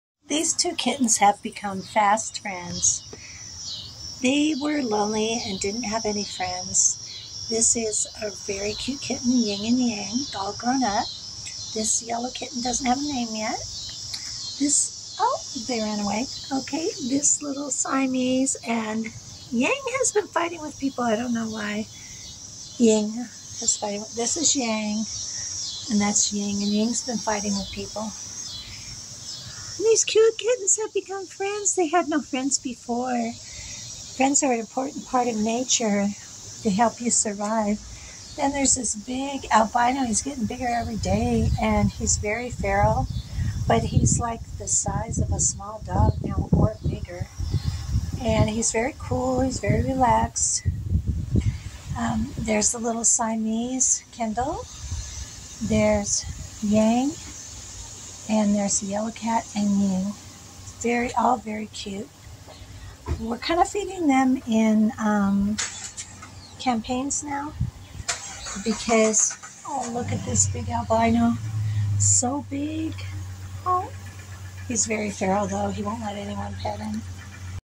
Kendall the kitten has made a friend. sometimes we don't have friends.
Making friends is a part of nature sometimes we don't have friends like little Kendall didn't have a friend before and now he's found a friend they will probably be friends for life.  friends can come and go so don't worry if you don't have a friend you'll make one just like little Kendall the cute fluffy beautiful Siamese kitten wants to go viral no one is so lovable no one deserves going viral like Kendall.  The cute yellow kittens I have healed and is getting big and strong. the large albino Siamese is very lazy he just lays in that round but he's very calm something is fighting with him we can see that but we don't know where or when. some of these kittens have beautiful blue eyes and the yellow kitten has gorgeous golden eyes.